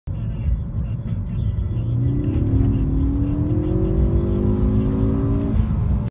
Focus st 2nd gear roll COBB stage 1 tune
2nd gear roll with only 3500 miles on so the tires are new and still can’t get traction on dry roads.  It’s putting some torque down!

You’ll notice the blinking yellow car for the traction control indicator the entire pull.  It’s located just below the analog speedometer.